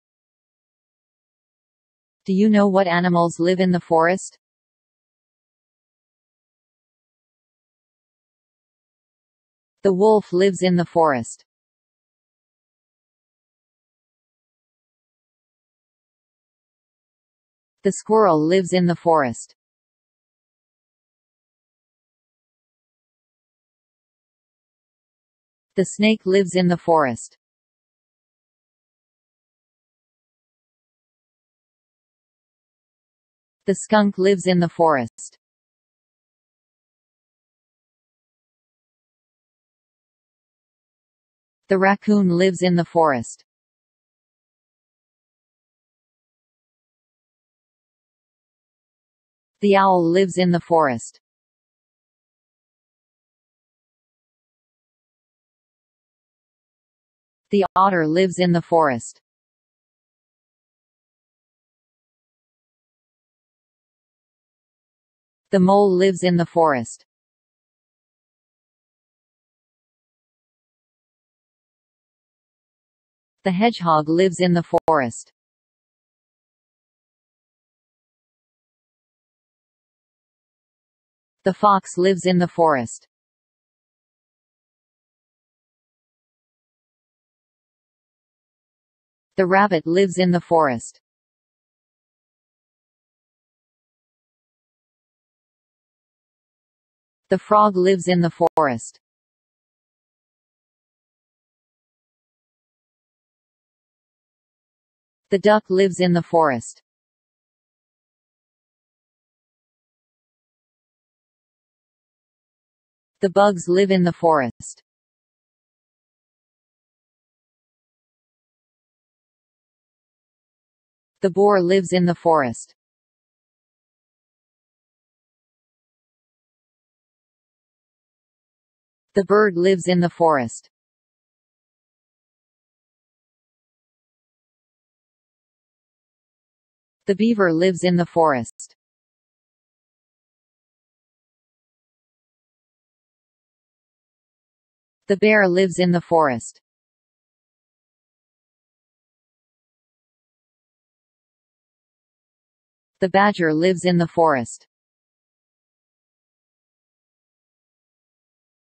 Do You Know What Animals Live In The Forest?
Nineteen Animals That Live In The Forest.